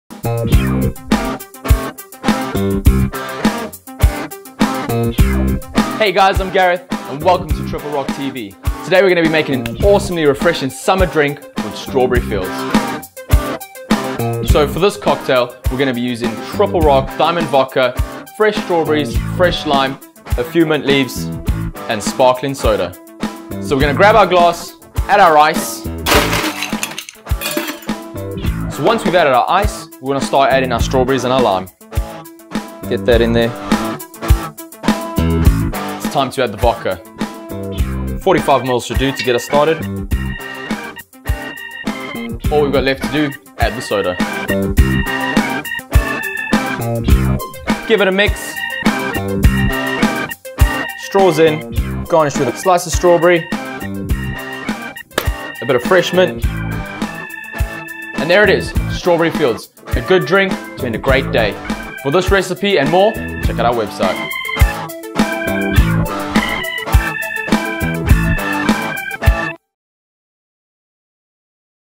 Gareth mixes the refreshing Strawberry Fields using Triple Rock Diamond Vodka
Strawberry Fields
For one person - fill a short glass with ice
- Add sliced strawberries
- Squeeze in fresh lime
- Add 45ml Triple Rock Diamond
- Top with soda water

Garnish with fresh mint